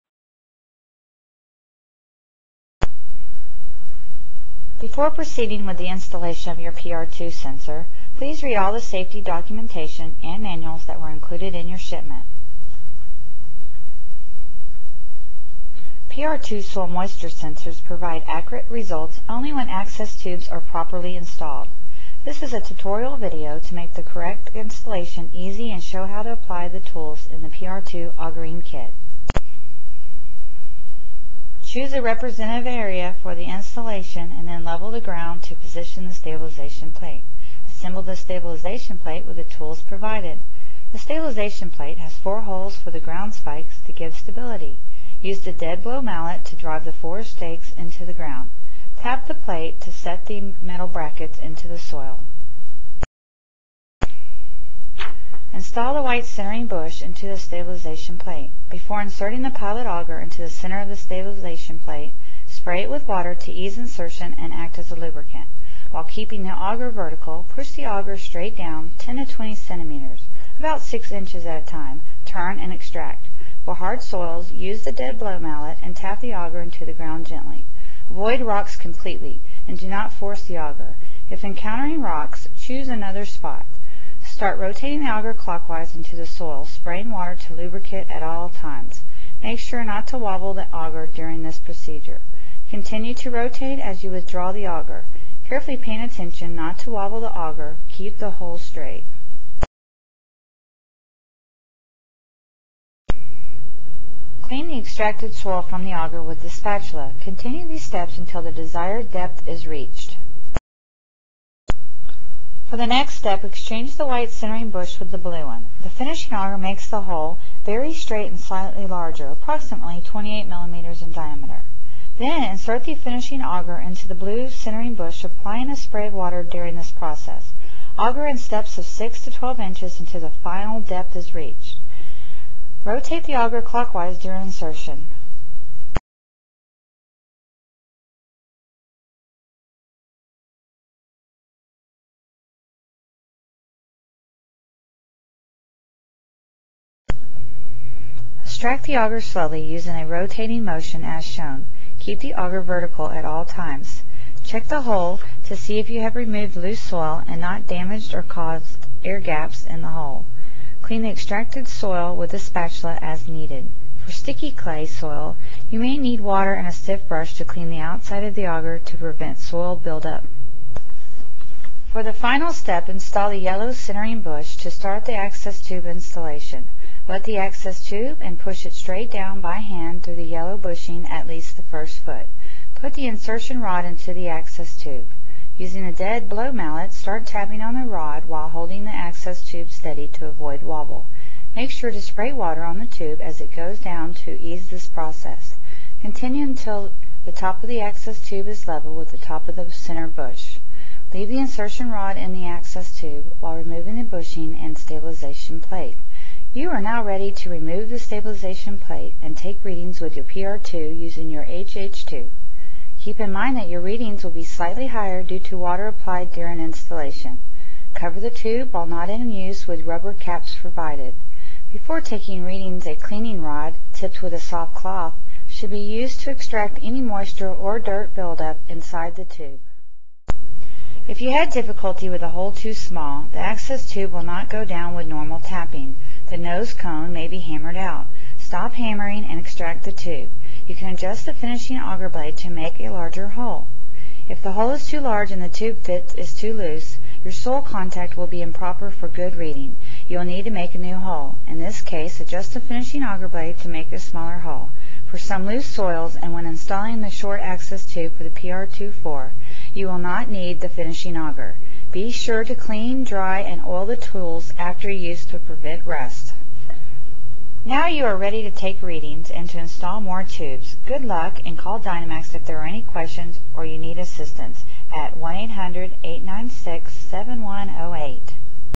PR2 Multi Depth Soil Moisture Sensor Installation
Installation video for the PR2 Portable SOil Moisture Profile Probe